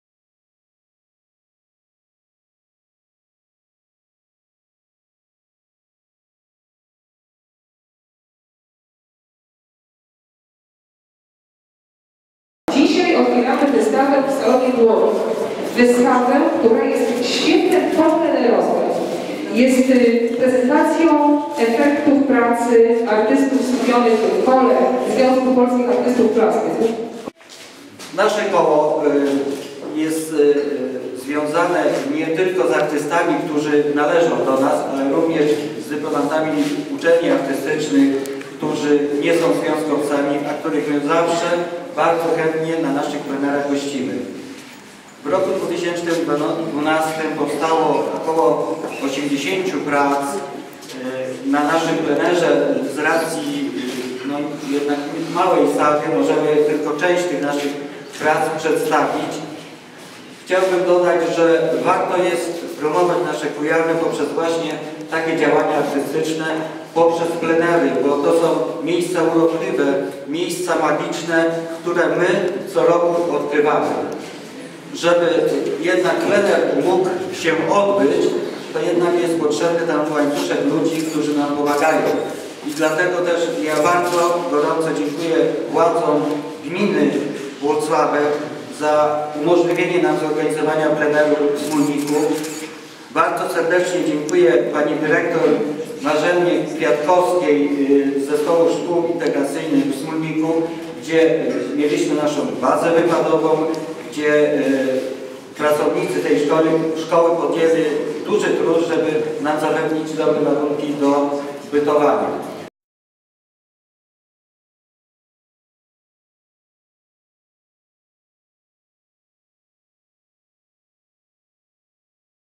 Włocławek. GSW: Wernisaż wystawy 'SMÓLNIK 2012' - 04.01.2013
W piątek, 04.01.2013 w Galerii Sztuki Współczesnej we Włocławku odbył się wernisaż poplenerowej wystawy prac artystów włocławskiego Koła ZPAP 'SMÓLNIK 2012'.

Swoje prace pokazali: Magdalena Bezat-Majeranowska, Andrzej Fronczak, Anna Grzelak, Jerzy Jabczyński, Zbigniew Jankowski, Mieczysław Janowski, Magdalena Kaczmarek, Iwona Kruszczyńska, Maria Sidor, Krzysztof Walczewski, Anna Wąsikiewicz.